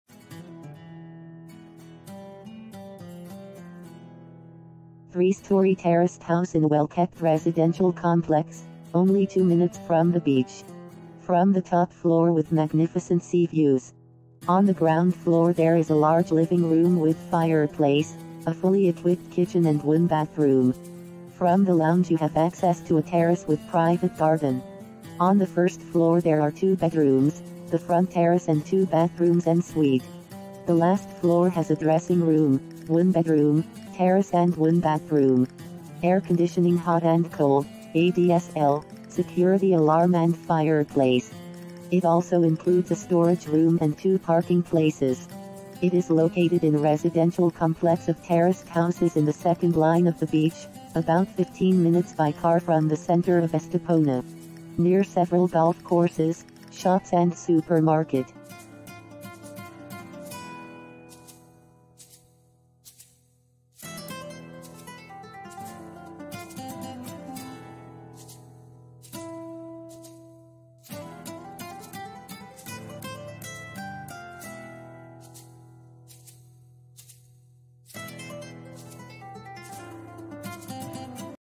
3 Bedrooms Townhouse in New Golden Mile, Costa Del Sol FOR SALE &euro; 430,000
3 Bedrooms Townhouse in New Golden Mile, FOR SALE &euro; 430,000
Three-storey terraced house in a well-kept residential complex, only 2 minutes from the beach.
From the top floor with magnificent sea views. On the ground floor there is a large living room with fireplace, a fully equipped kitchen and 1 bathroom. From the lounge you have access to a terrace with private garden. On the first floor there are 2 bedrooms, the front terrace and 2 bathrooms en suite. The last floor has a dressing room, 1 bedroom, terrace and 1 bathroom. Air conditioning hot and cold, ADSL, security alarm and fireplace. It also includes a storage room and 2 parking places. It is located in a residential complex of terraced houses in the second line of the beach, about fifteen minutes by car from the centre of Estepona. Near several golf courses, shops and supermarket
Covered Terrace;Near Transport;Storage Room;Ensuite Bathroom;Marble Flooring
Beachside;Close To Golf;Close To Shops;Close To Sea;Close To Schools;Urbanisation